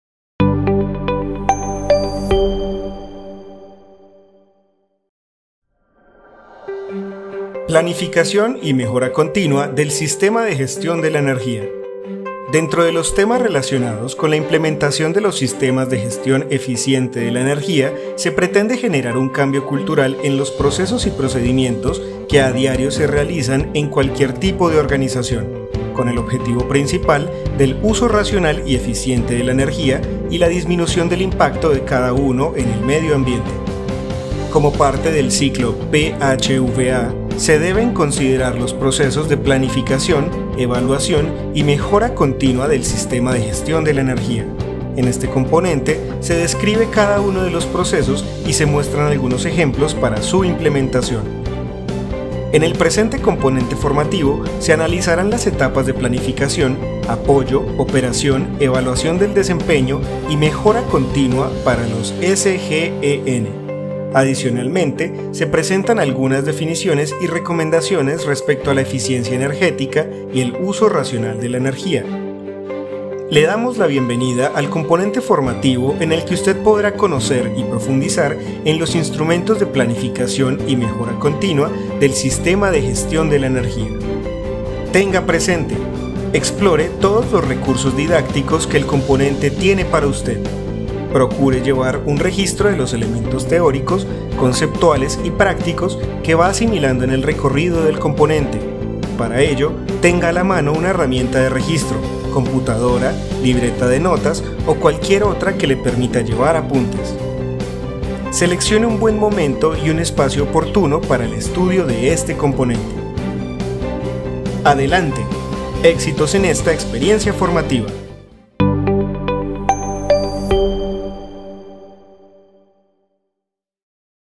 Planificación y mejora continua del Sistema de Gestión de la Energía
Este video hace parte del Tecnólogo Gestión Eficiente de la Energía. Para ver todo el programa de formación te invitamos a estar atento a las fechas de inscripción